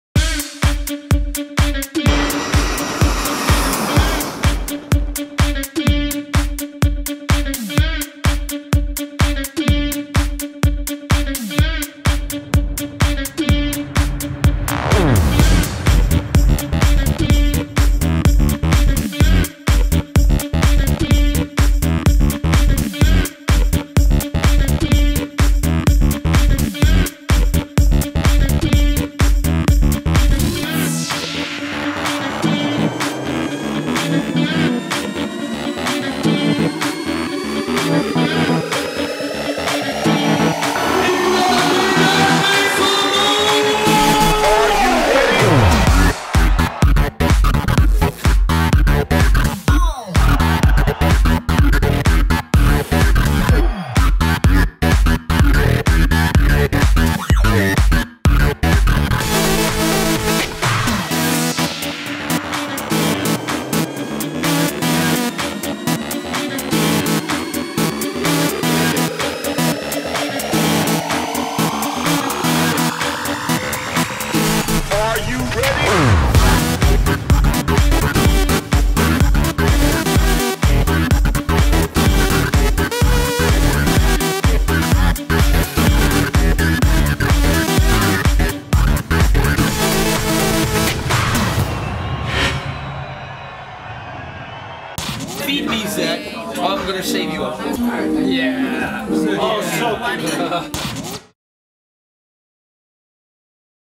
GREGORI KLOSMAN @ PACHA IBIZA
Summer 2013, GREGORI KLOSMAN plays for the first time in the legendary PACHA IBIZA alongside his Dirty Dutch fellow DJ's : Chuckie, Glowinthedark but also a very special guest named Zedd. Check it out...